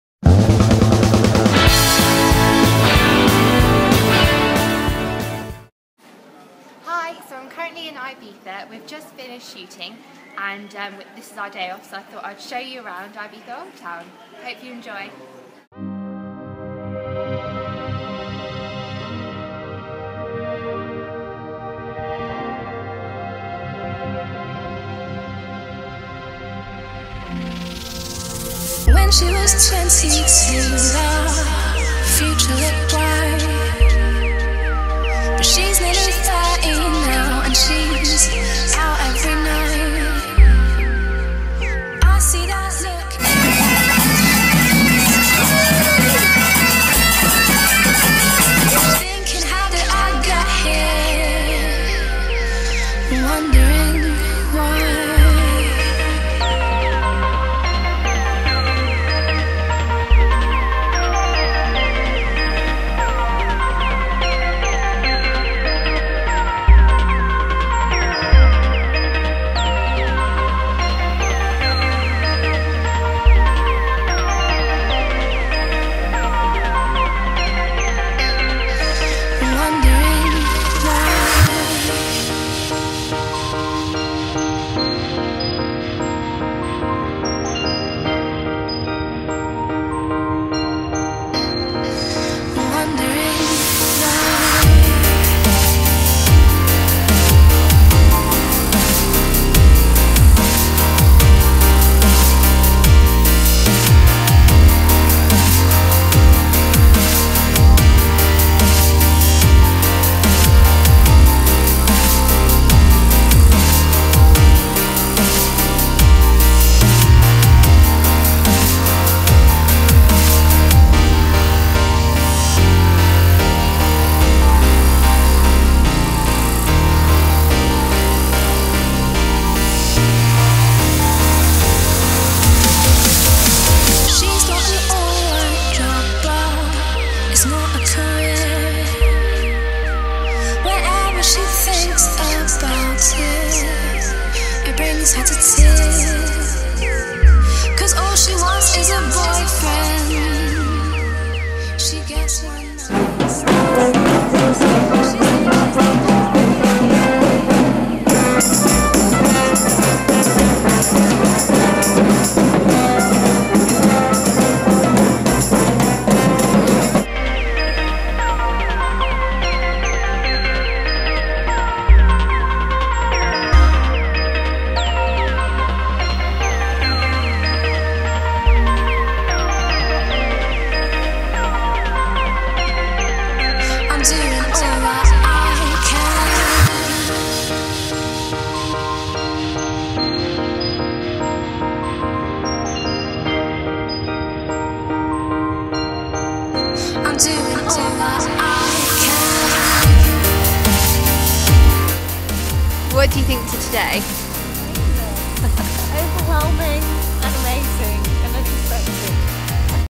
IBIZA VLOG | SARAH-ROSE GOES
I hope you guys enjoy my little montage of mine and Lauren's day in Ibiza Old Town! We were in Ibiza working on several shoots for a popular weekly magazine and made the most of our day off, exploring the Medieval Festival and local markets.

LAUREN'S TWITTER: @LaurenNmakeup